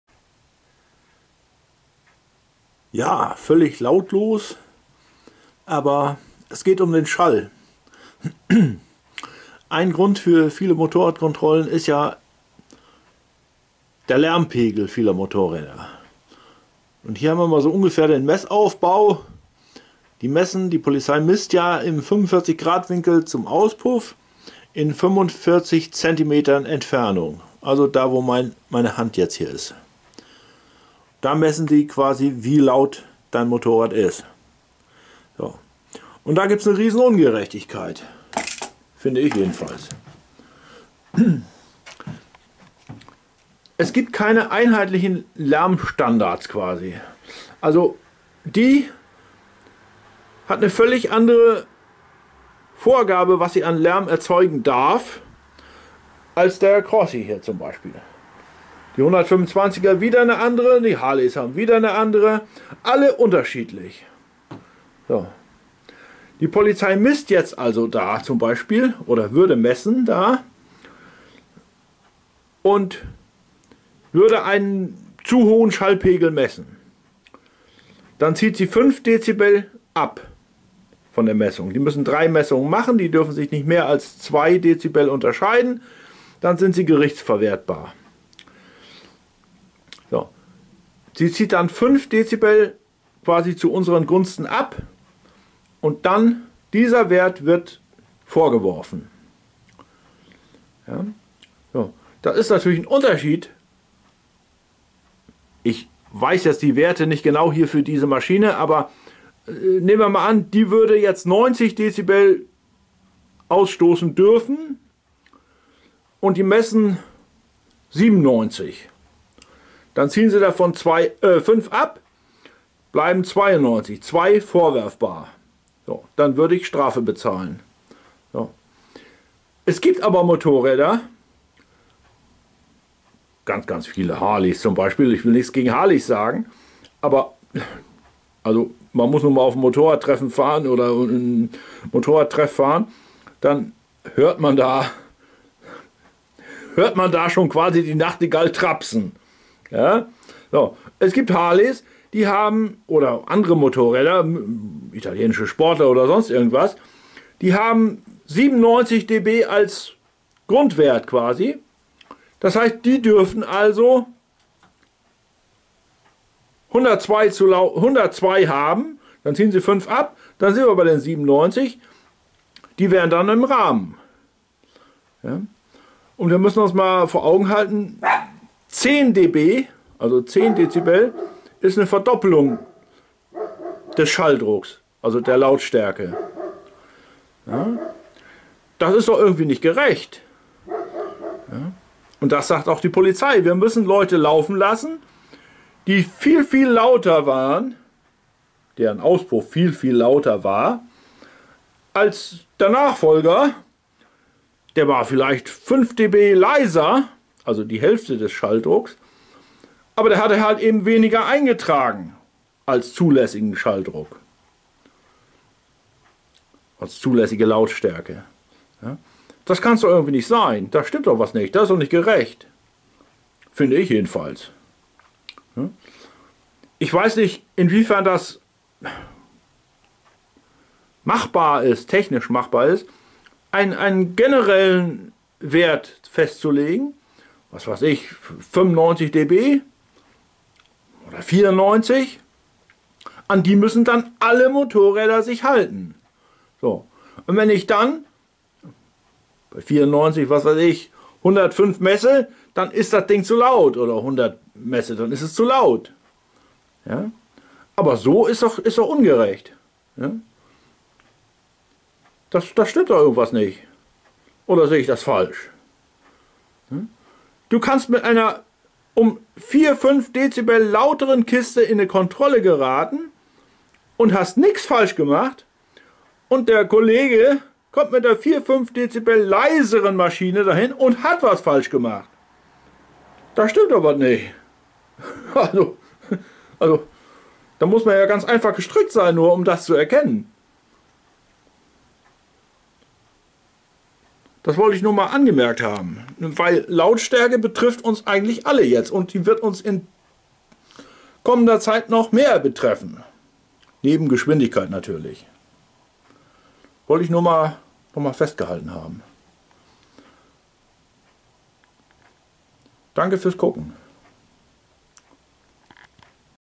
Dezibel-Roulette ...es geht um Sound und Lärm ....bei Motorrädern😎
Ich bin ein großer FAN   VON SOUND (!)  bei Motorrädern !

OHNE   oder   zu Tode  gedämpft, würde mir  was Entscheidendes fehlen.
Was ich NICHT Mag , sind "mehrerlei Maßstäbe"! .
Darum gehts   hier.